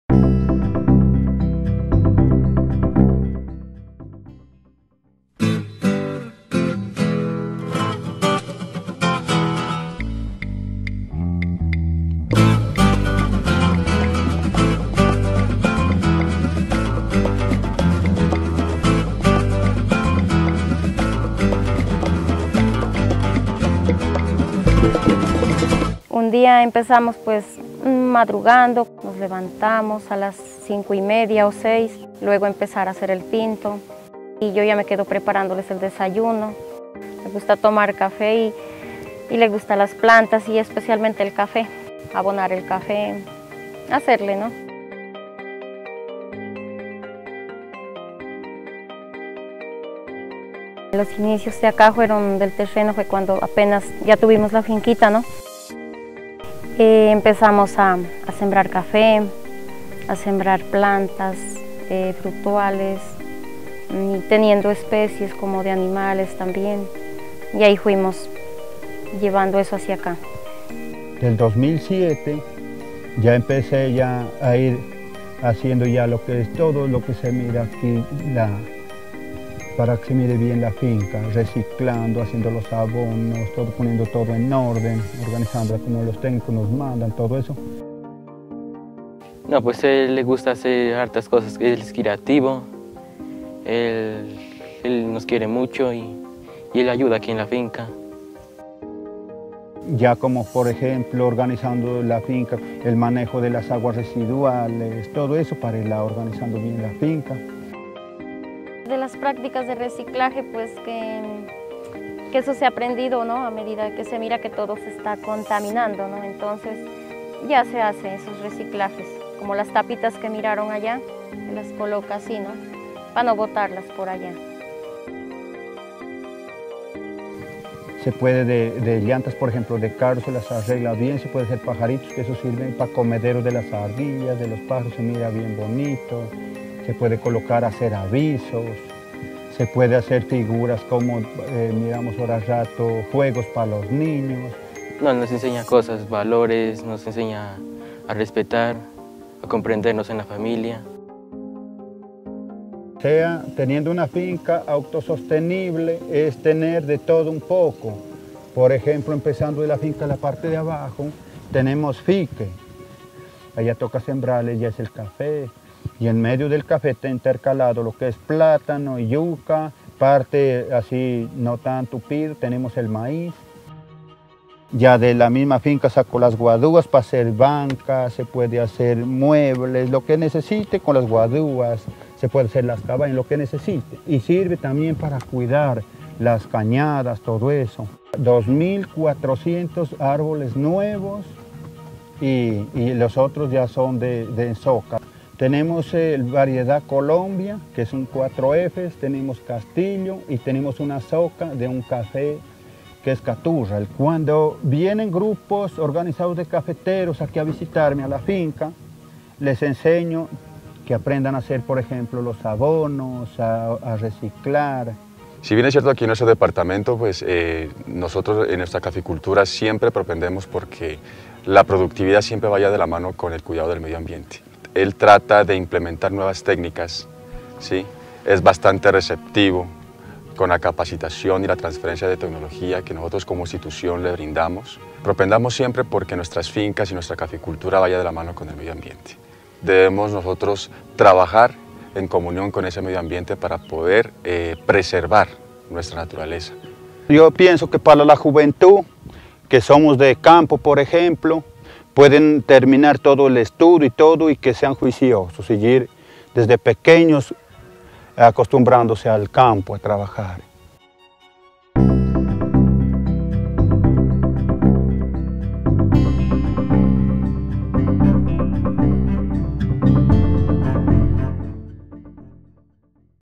Jairo Agreda, cafetero Nariño
Héroes de la Caficultura 2017 - Un homenaje de los caficultores para los caficultores.

Jairo Agreda, caficultor de Nariño, un héroe que con ingenio reutiliza residuos reciclables en su finca para contribuir con el medio ambiente.